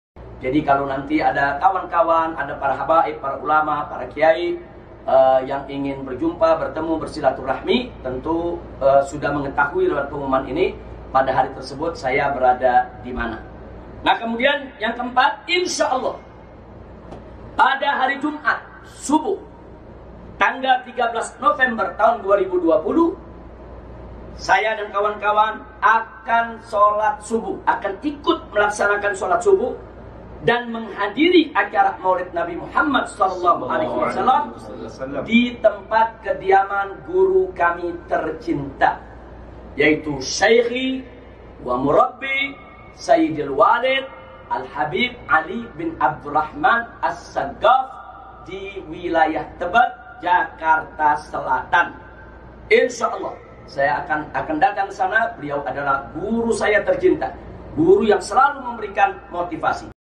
Virall !! Alhamdulillah Habib Rizieq Akan Pulang Ke Indonesia
Alhamdulillah Akhirnya Imam besar Umat islam Indonesia Habibana Muhammad Rizieq bin husein Syihab Akan Pulang Ke Indonesia, dan Pada tanggal 13 November nya Akan hadir si Acara Maulid Syaidil walid Habib Ali Bin Abdurrohman Assegaf

Habib Muhammad Rizieq Bin husein Syihab
Habib Ali bin Abdurrohman Assegaf
Habib Umar bin Hafidz
Habib Syech Assegaf
Habib Hasan bin Jafar Assegaf